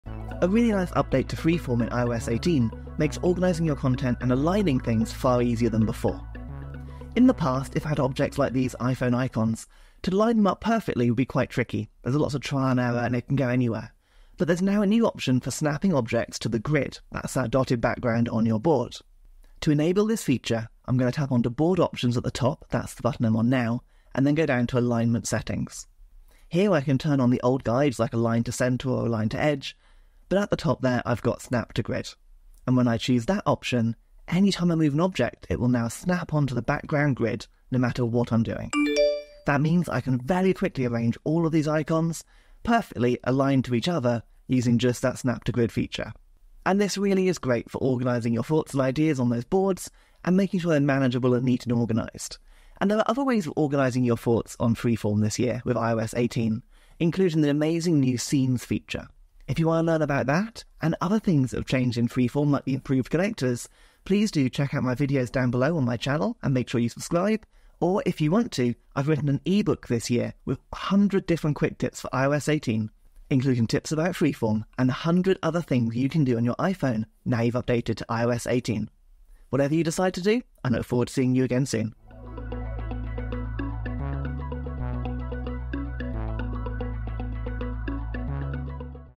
iOS 18 Freeform: Perfect Alignment with Snap to Grid
Discover the new “Snap to Grid” feature in Freeform with iOS 18, making it easier than ever to align and organise your content. This video demonstrates how to quickly and neatly arrange objects on your Freeform boards, ensuring a tidy workspace.